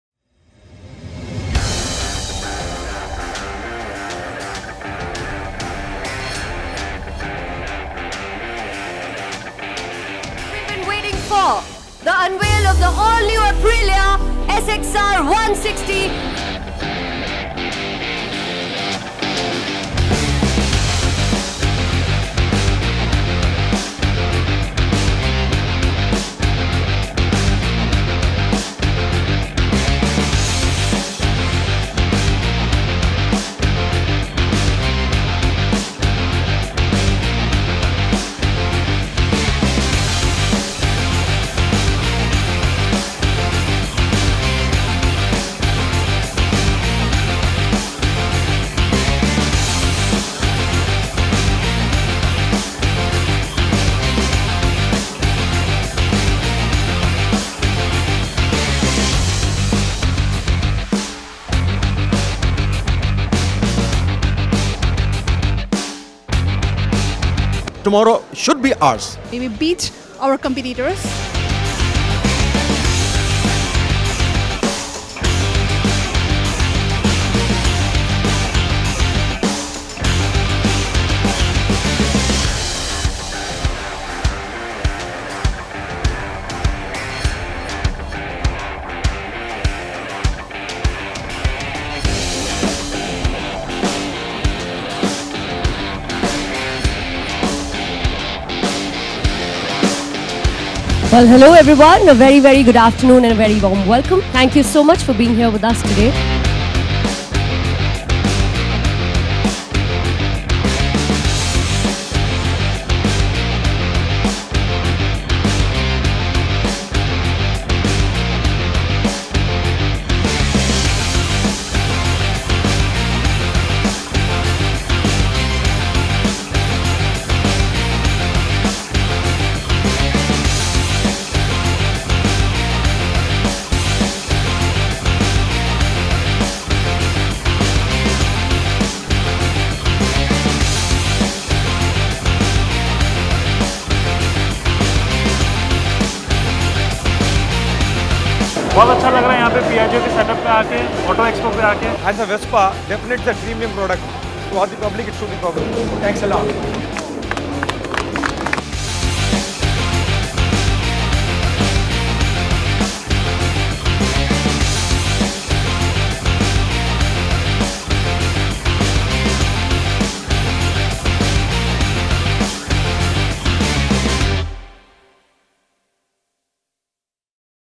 Piaggio Auto Expo 2020
The company with a world class reputation in the light mobility segment, made a great impact at Auto Expo 2020 with an award-winning launch and showcase of their varied range of products for the 9 days exhibition. Diego Graffi, CEO and MD, Piaggio Vehicles unveiled the all new Aprilia SXR 160 and showcase the Vespa Elettrica as an inspiring electric mobility solution.

The Pavilion showcased a display of 15 exquisite models including the limited-edition Vespa Racing Sixties, Vespa 2020 Facelifts, Aprilia SR 160 and Aprilia Storm 125 with disc brakes.  Piaggio Pavilion won two awards at the Auto Expo 2020. The Best Pavilion in the Two-wheeler category and Aprilia SXR 160 won People’s choice award for the best two-wheeler.

Wizcraft designed, fabricated and managed the Piaggio Pavilion at the Auto Expo 2020. The Piaggio Pavilion being 456 square meter in size was perfectly placed with a two side open view, enhancing the look and feel. The design focused on the vast expanse and uncluttered display of 15 Piaggio’s premium brands. The design was created to highlight specific brand positioning, appeal, design, ergonomics, features and comfort.  Seamless integration and innovative display of varied Piaggio brands with different brand positioning and ethos came alive.

‘I am the Power’ quoted by the all new electric, futuristic Vespa’s Elettrica was Piaggio’s futuristic electric mobility solution. Elettrica was featured on a podium designed as per international standards. The fascinating design, elevated the brand look and put a spotlight on the trend-setter brand ‘Elettrica’.